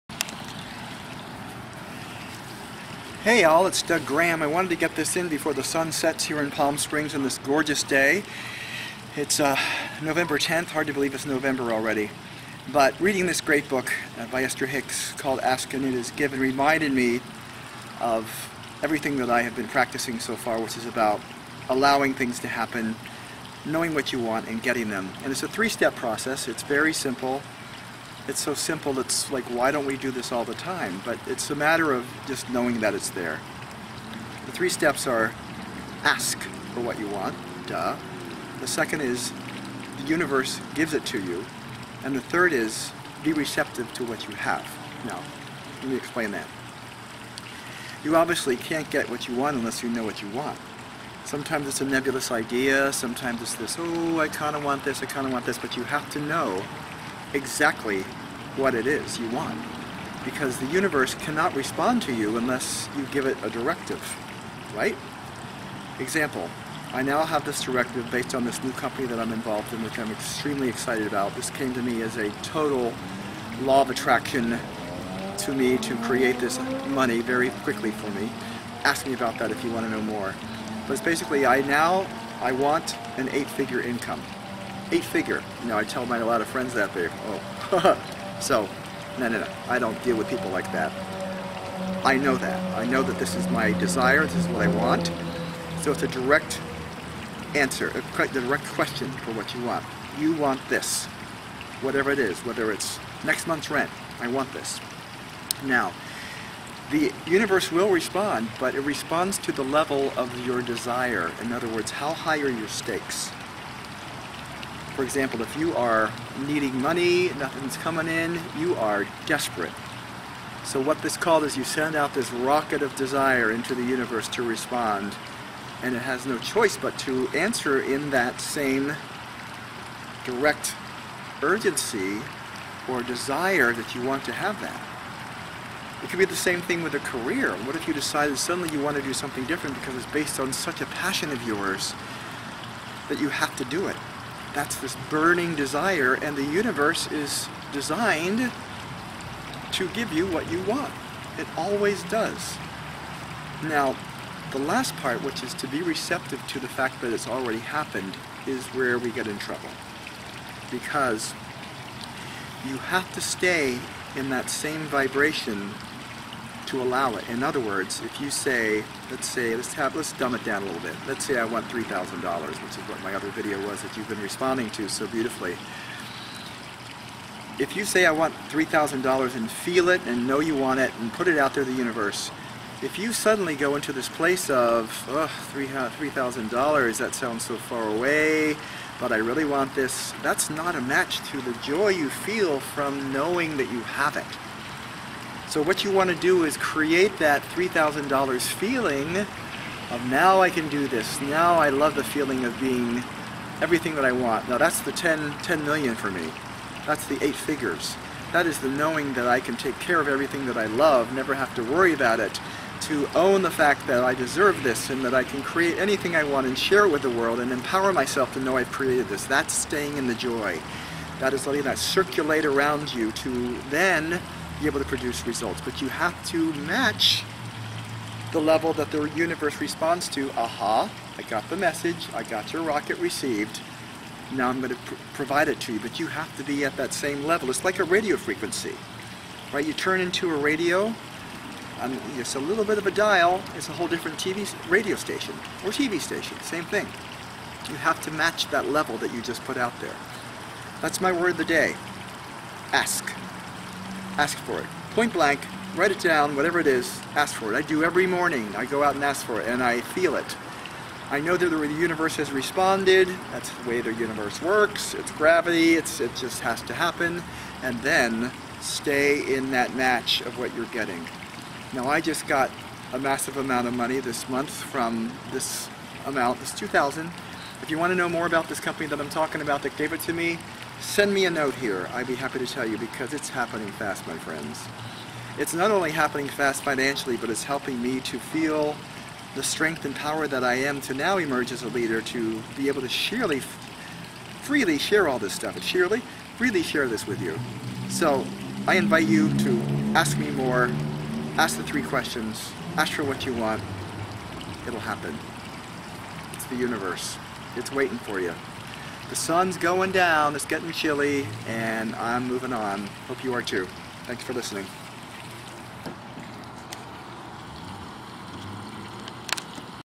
Three Easy Steps to Manifesting Anything You Want
It really could not be any easier than this. But the key is...You have to do it! Watch, feel and take action. Let me know what shifts for you.